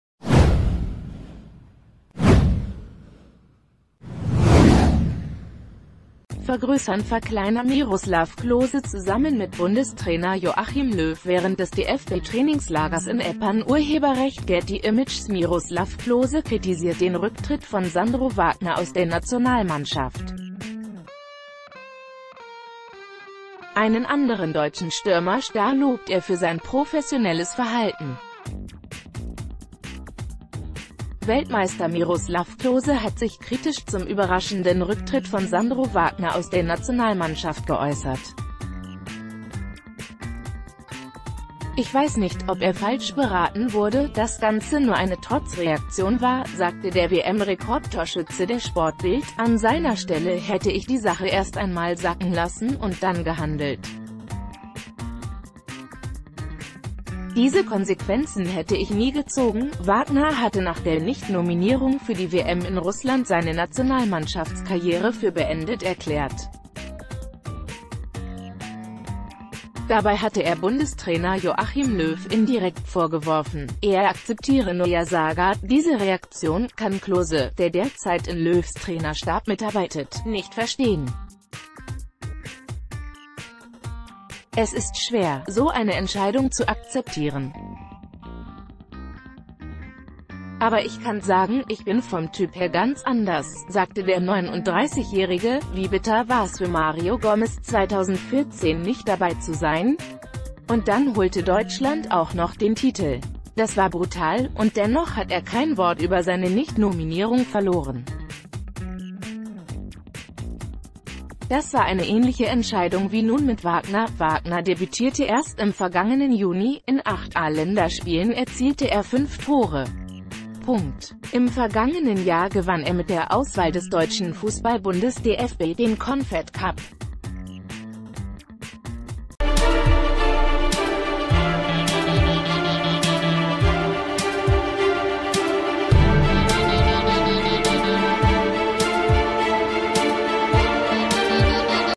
Miroslav Klose kritisiert Rücktritt von Sandro Wagner aus der Nationalmannschaft
vergrößernverkleinern Miroslav Klose zusammen mit Bundestrainer Joachim Löw während des DFB-Trainingslagers in Eppan © Getty Images Miroslav Klose kritisiert den Rücktritt von Sandro Wagner aus der Nationalmannschaft. Einen anderen deutschen Stürmer-Star lobt er für sein professionelles Verhalten. Weltmeister Miroslav Klose hat sich kritisch zum überraschenden Rücktritt von Sandro Wagner aus der Nationalmannschaft geäußert. "Ich weiß nicht, ob er falsch beraten wurde, das Ganze nur eine Trotzrea...